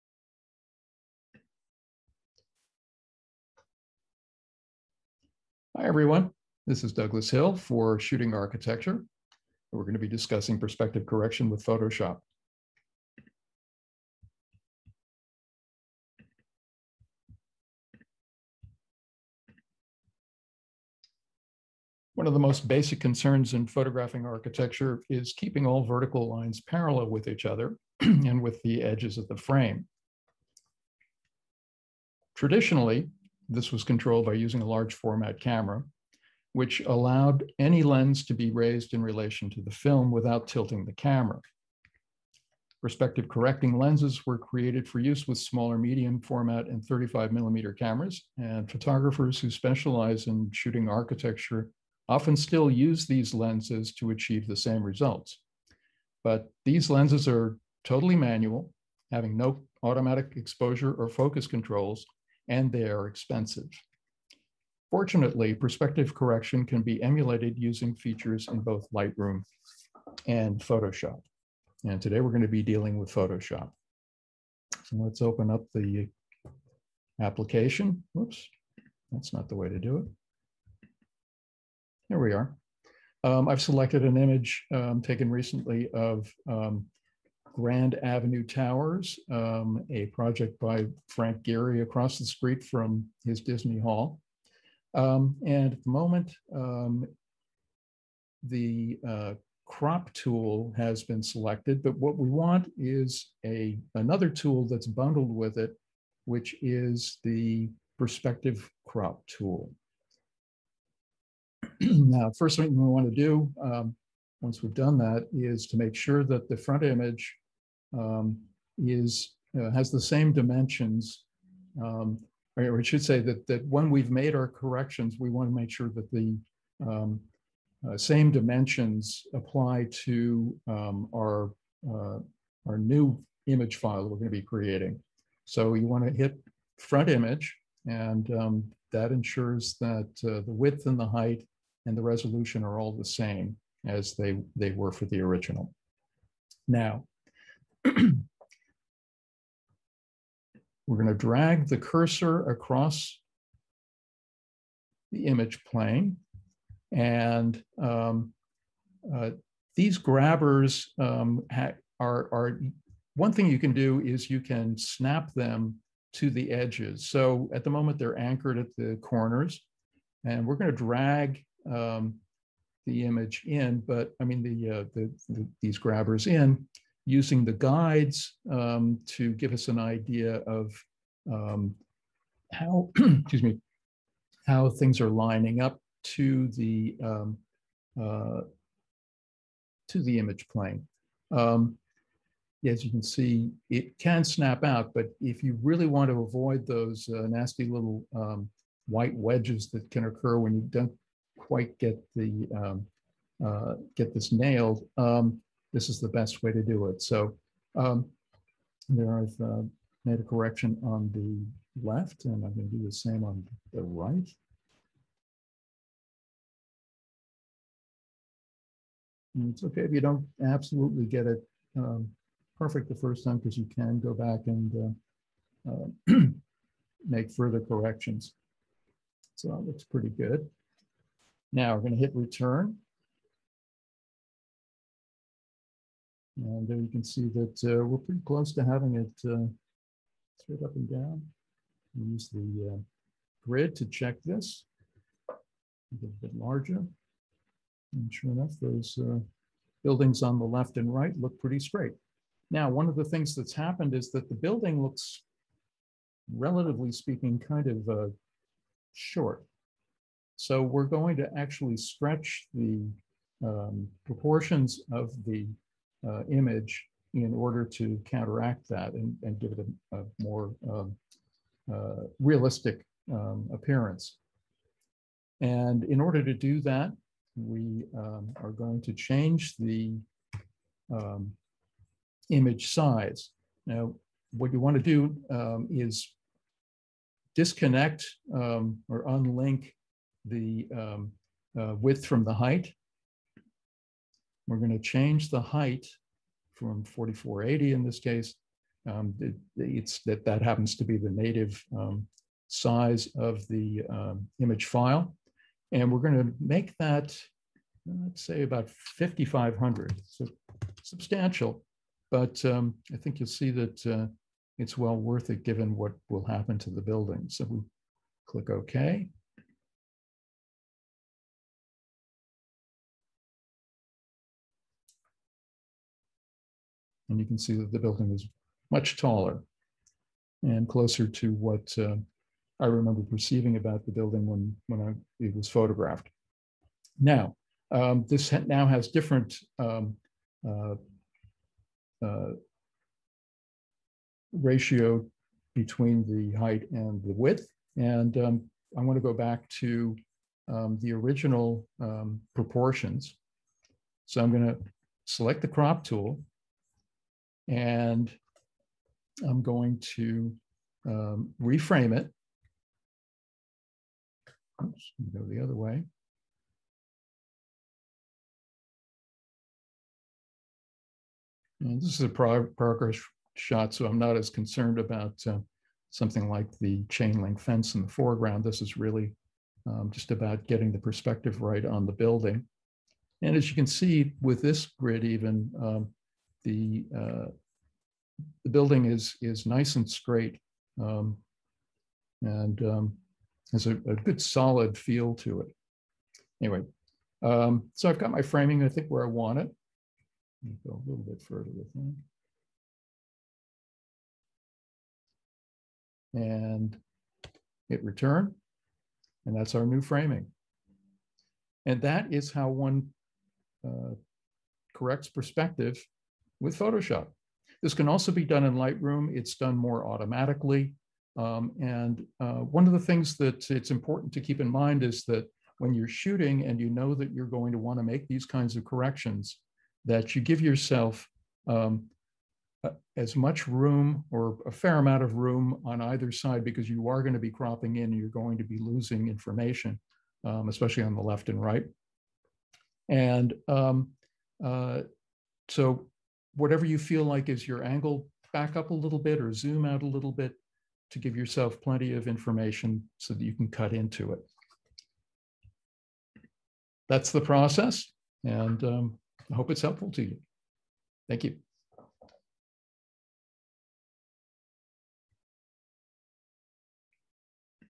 Shooting Architecture: Perspective Correction with Photoshop
A demonstration by Douglas Hill of how to correct perspective in an architectural photograph using Adobe's Photoshop.